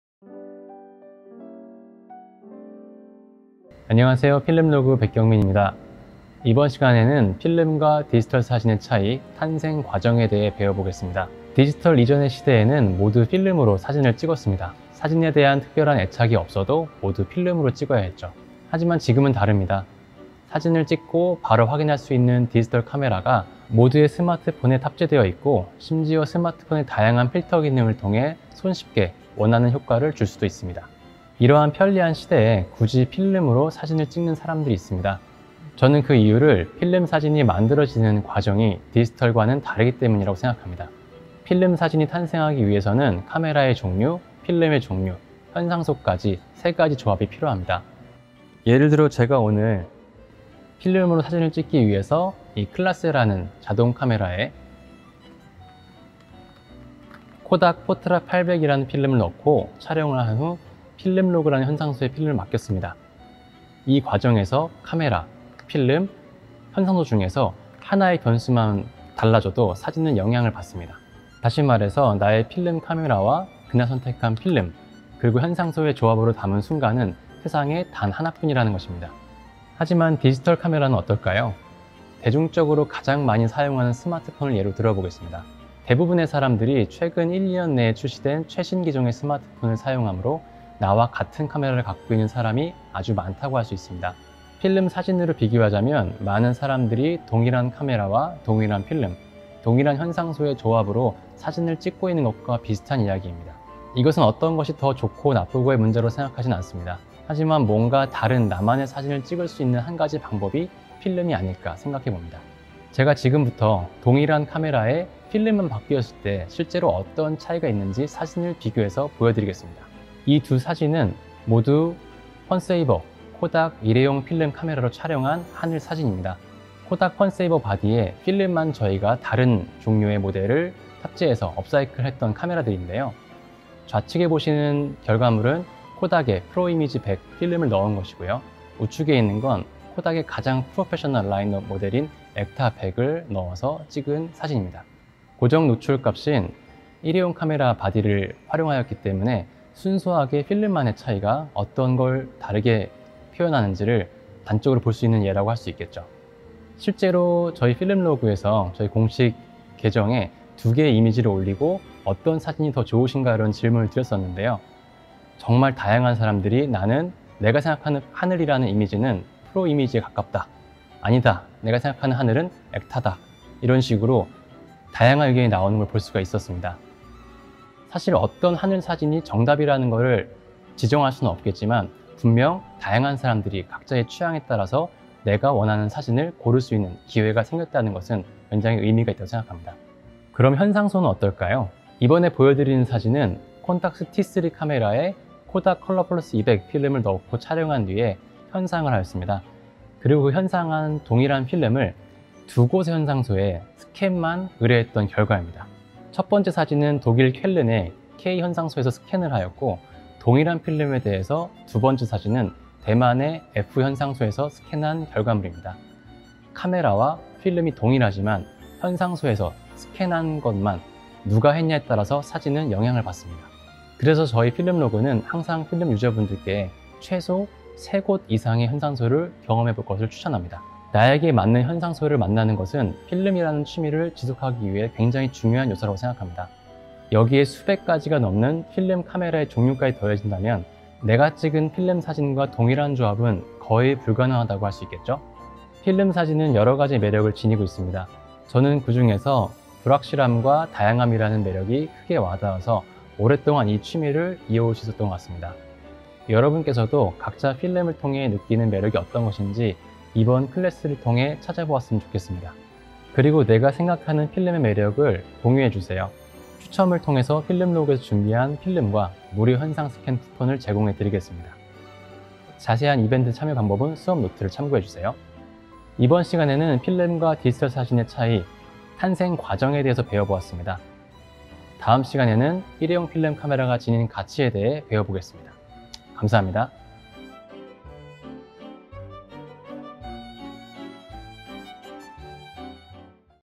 [SDF x CLASS101] 1-2. 필름과 디지털 사진의 차이 : 과정
CHAPTER 1 - 다시 시작된 필름 문화
그 두 번째 이야기! 이번에는 탄생 과정에 있어서 필름과 디지털의 차이가 무엇인지 알아보아요.